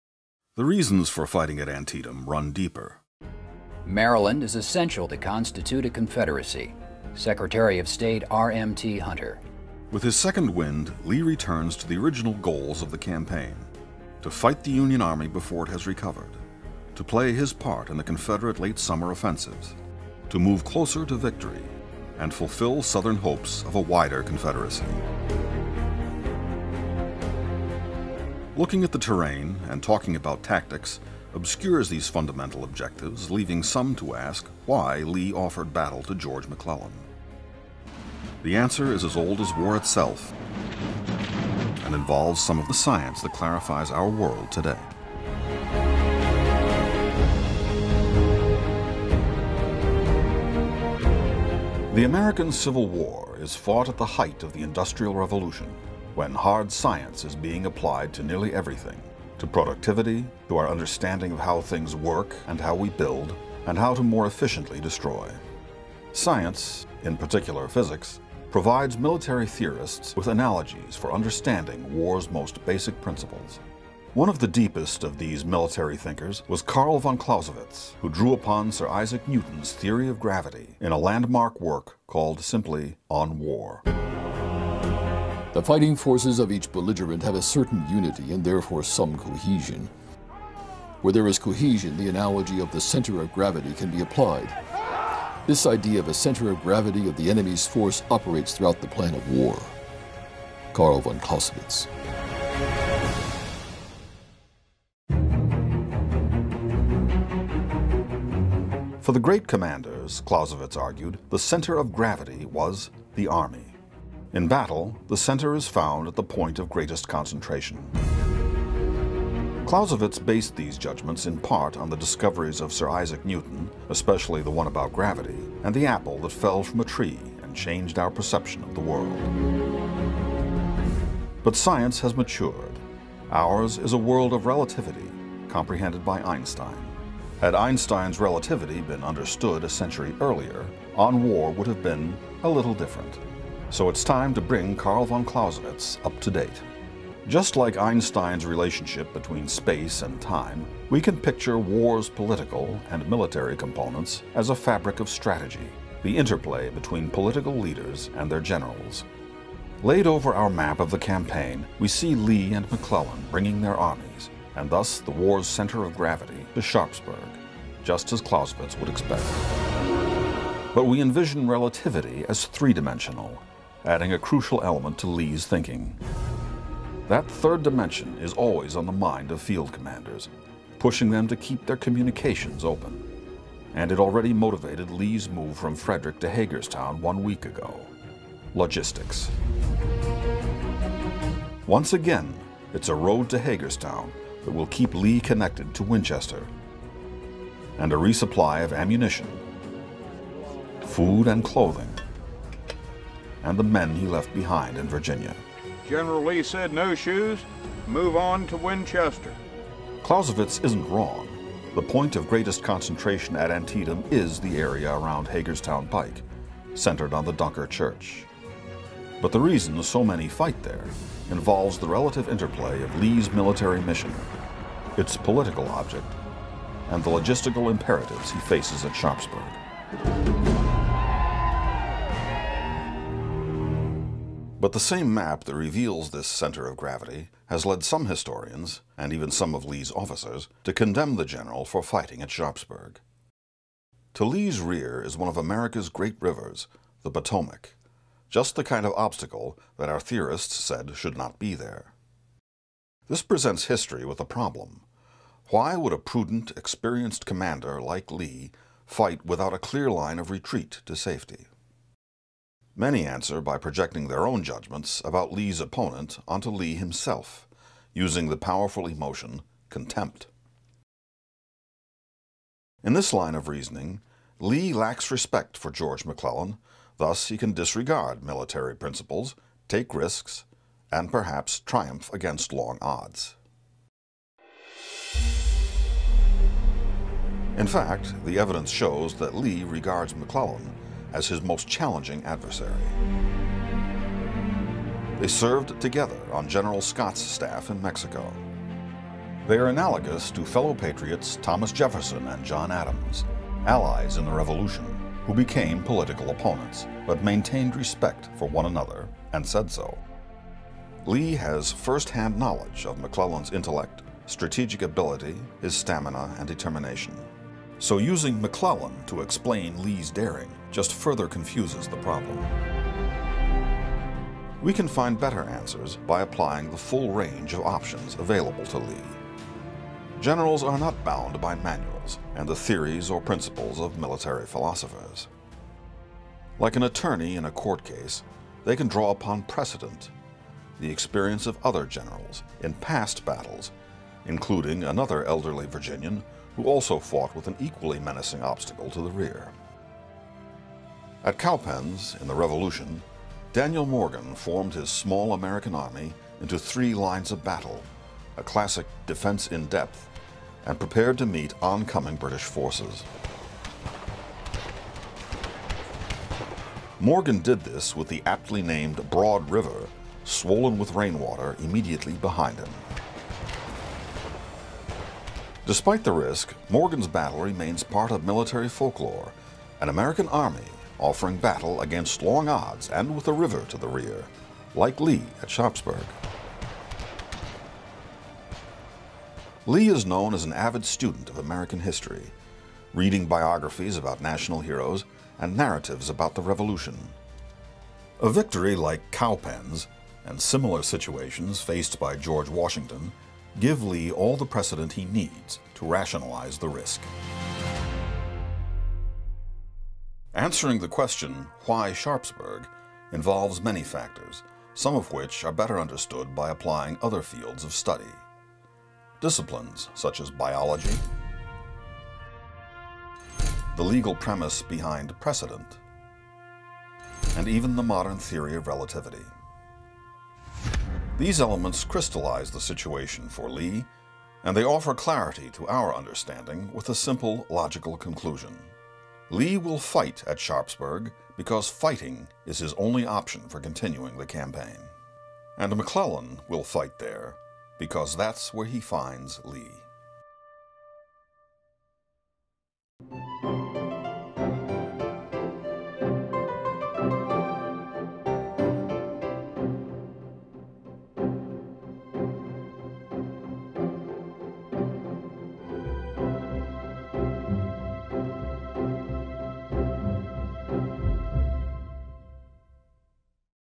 ANTIETAM, Mini-series; 5/8: “Antietam According to Clausewitz, Newton, and Einstein”
What would Carl von Clausewitz have said about the Battle of Antietam; and since Clausewitz based so much of his theory on the principals of Sir Isaac Newton, what would Newton have said about it?  We can only surmise – but since Albert Einstein brought us to a greater understanding of our world, a better question is “How can we apply Einstein’s theory of relativity to better understand the Maryland Campaign?”